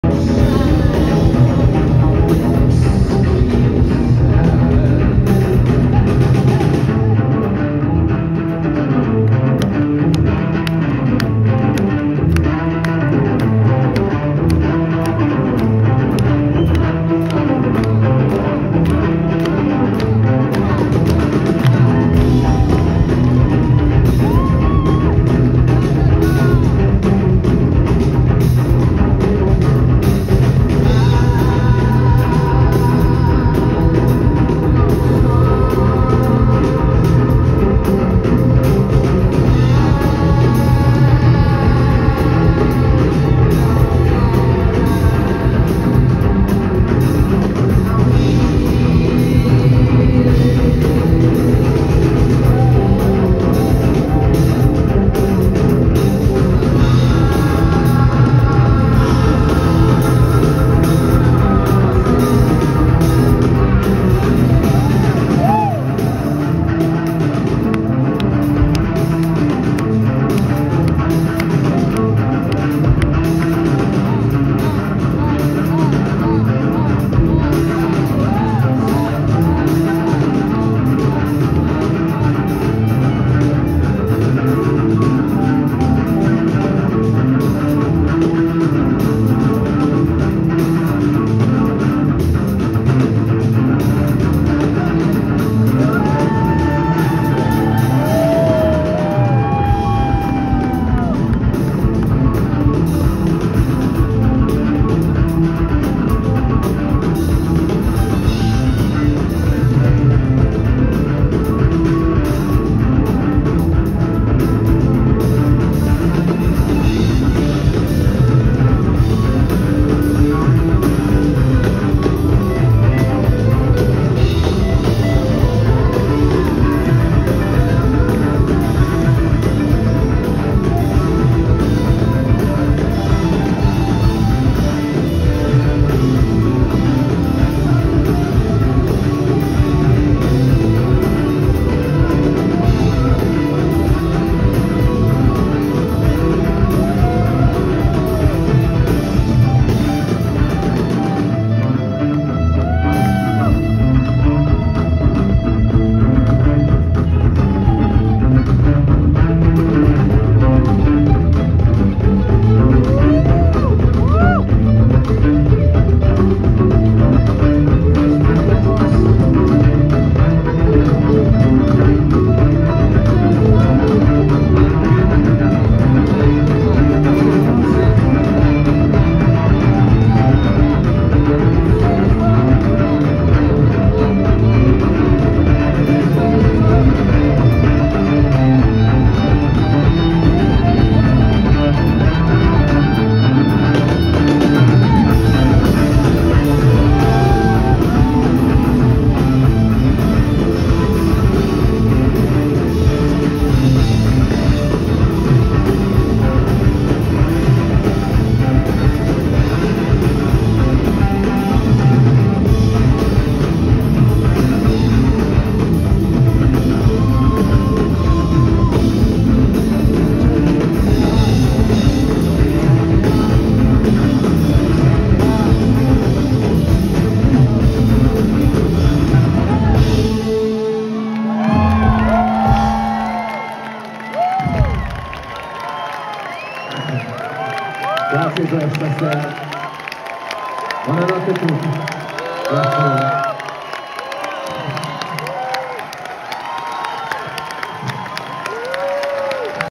The Smile @ Fabrique, Milano, July 14, 2022
The Smile in concert at the Fabrique in Milano, July 14, 2022.